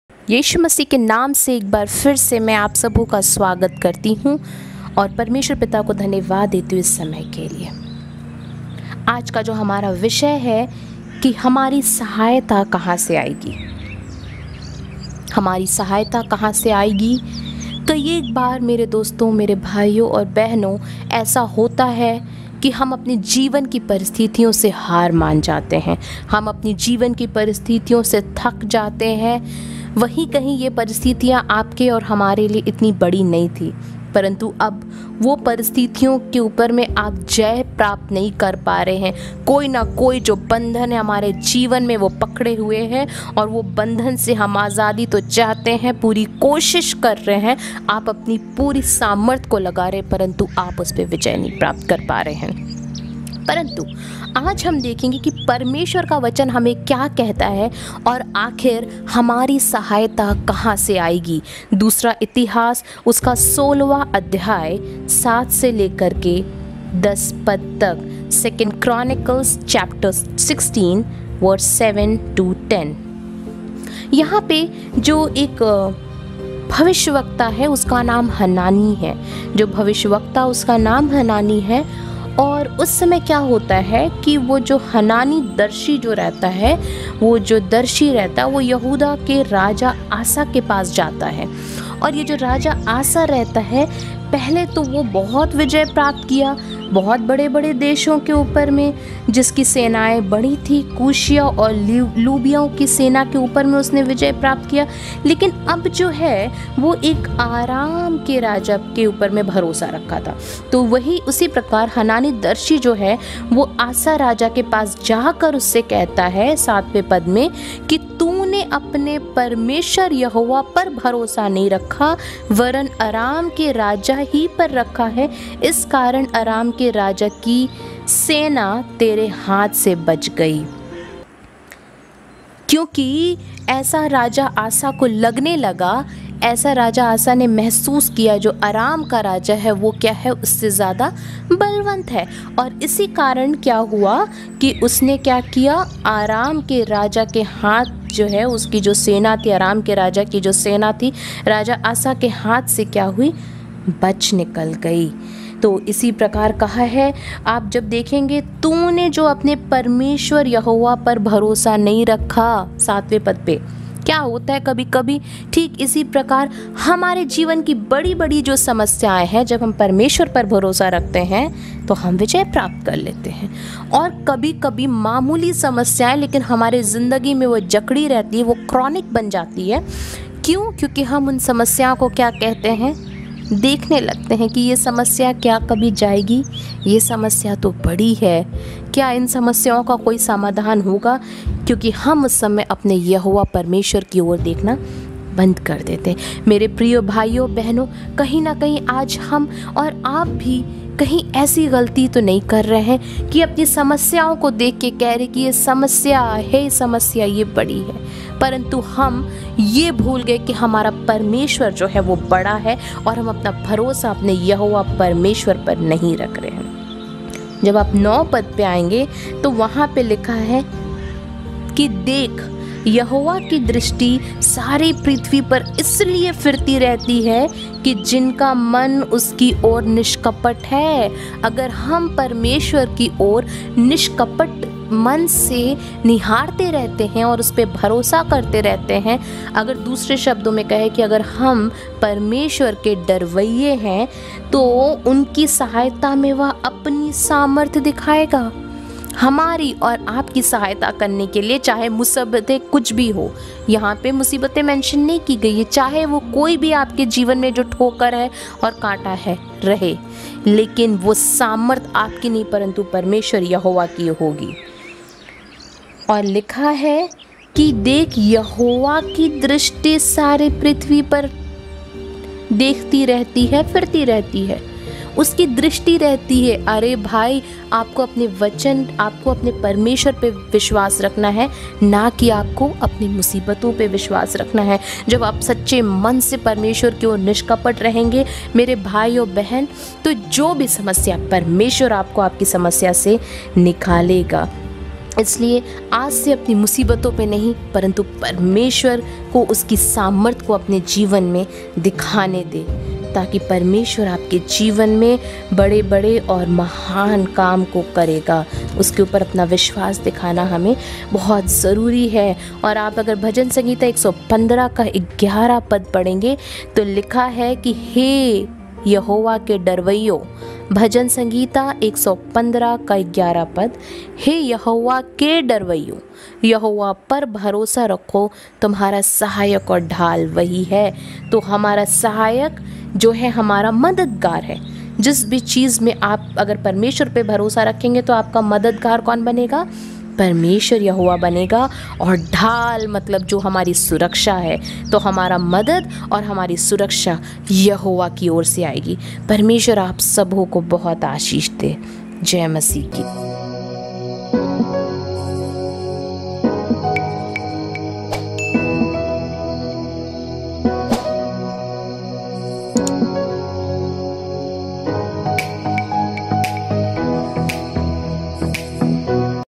Where does my help comes from
This short sermon is based on famous Psalms 121 which says,  Where does my help comes from ( By Sis Vandana R Rassmann ) here she has spoken about that we must lean on God in our time of distress and turmoil. God is our refuge and strength on whom we must cling to, because he has promised us that he will never leave nor forsake us.

May Our good Lord Bless each and everyone of us For watching it.
Blessings .